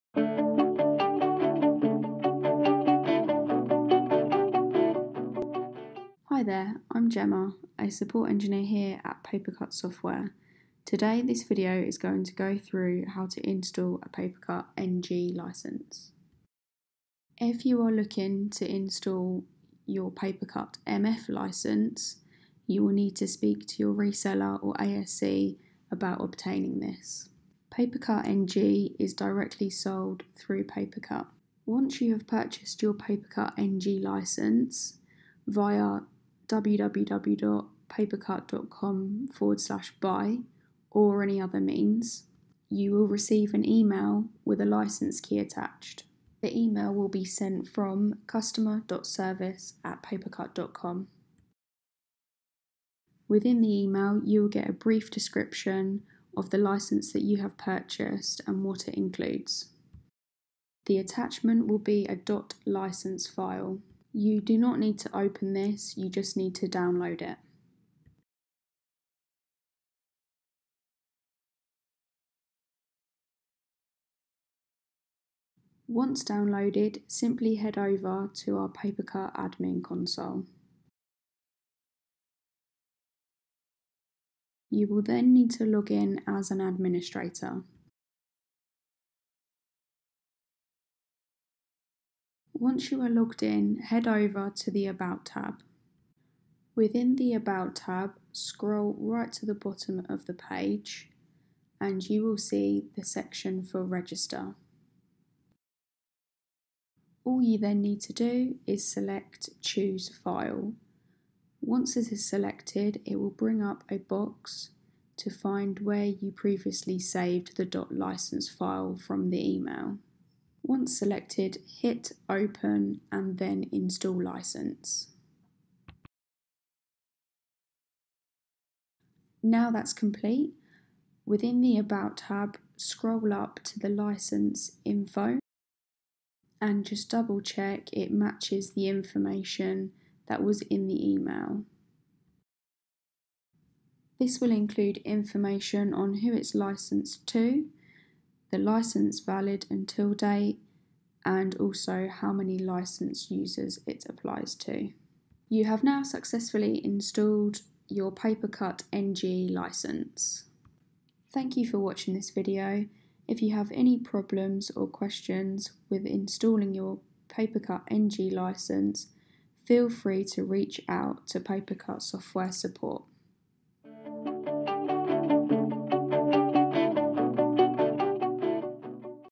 How to Install a PaperCut NG License
This video walks you thought the steps on How to Install a PaperCut NG License.